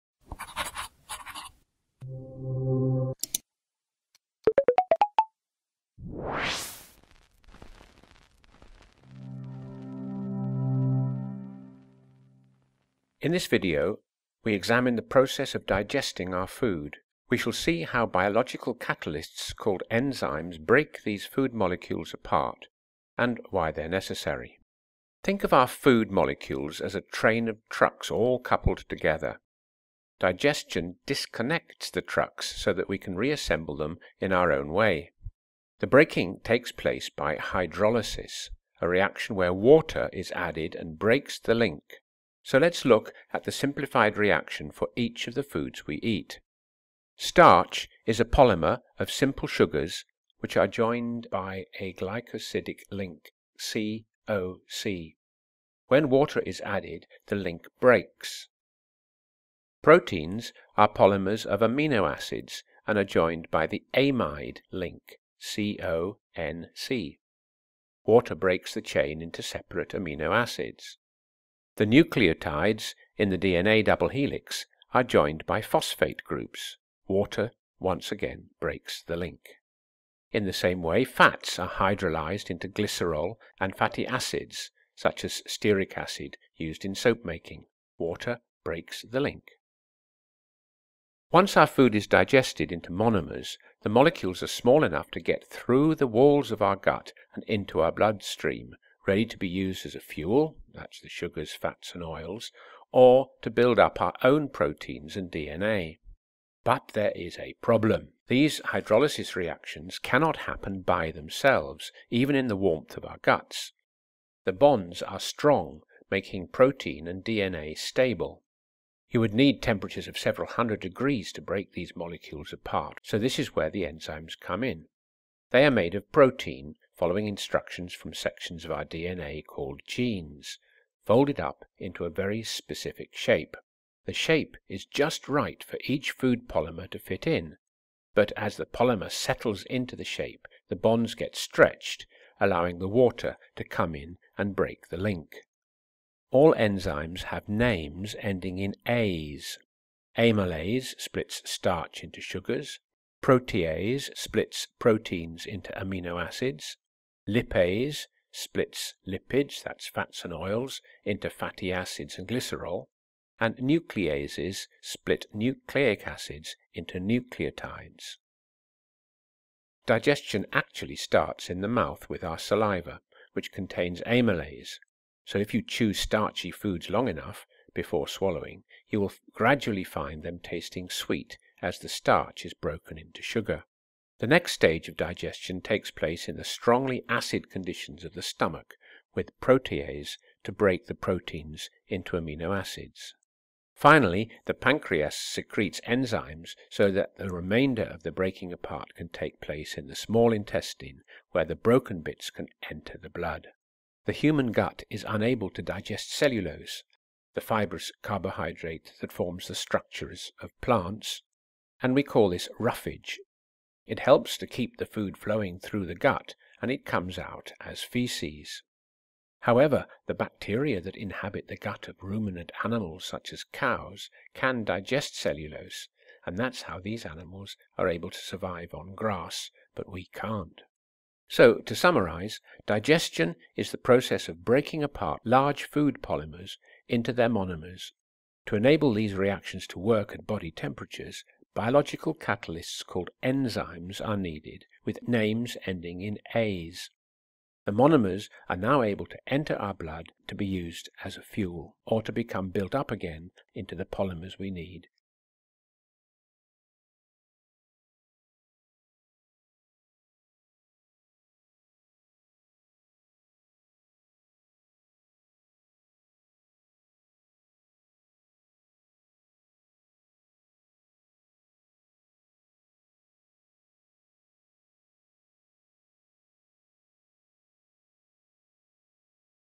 Digestion By Enzymes | Organic Chemistry | FuseSchool
In this video we examine the process of digesting our food. We shall see how  biological catalysts called enzymes break these food molecules apart and why they are necessary.

Think of our food molecules as a train of trucks all coupled together. Digestion disconnects the trucks so we can re-assemble them in our own way.

The breaking takes place by hydrolysis – a  reaction where water is added and breaks the link. Let’s look at the simplified reaction for each of the foods we eat:

Starch is a polymer of simple sugars which are joined by a glycosidic link C–O-C . When water is added the link breaks.

Proteins are polymers of amino acids are joined in by the amide C-O-N-C link. Water breaks the chain into separate amino acids.

The nucleotides in the DNA double helix are joined by phosphate groups. Water breaks the link.

In the same way fats are hydrolysed into glycerol and fatty acid, such as stearic acid used in soap making! Water breaks the link

Once our food is digested into monomers the molecules are  small enough to get though the walls of our gut and into our blood ready to be used as fuel (sugars, fats and oils ) or to build up our own proteins and DNA.